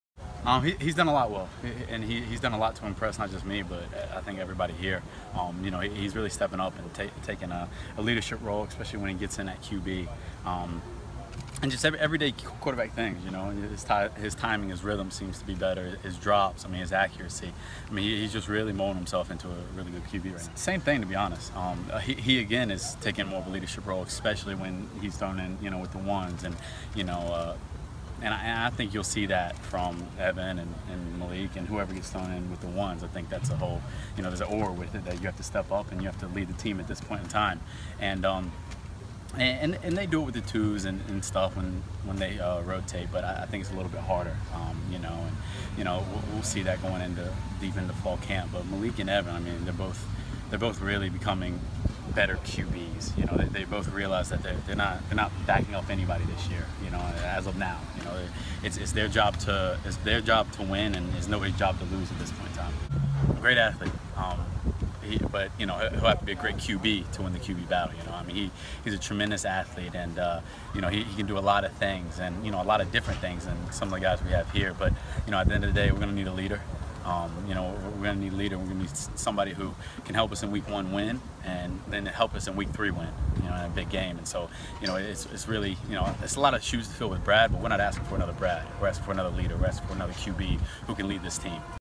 Braxton Berrios on Miami Hurricanes QB battle
Miami wide receiver Braxton Berrios on quarterbacks Evan Shirreffs, Malik Rosier and N'Kosi Perry, three contenders to take over for departed Hurricanes passing leader Brad Kaaya. (Matt Porter/The Palm Beach Post)